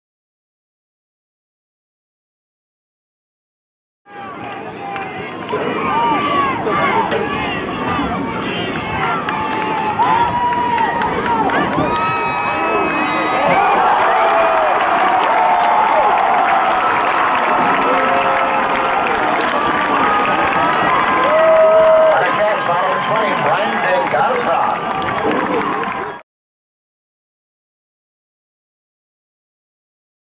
RGV All Star Game: Justin Owens TD pass to Brandon Garza
Nikki Rowe's Justin Owens 11 yard TD pass to Sharyland's Brandon Garza for the West Side.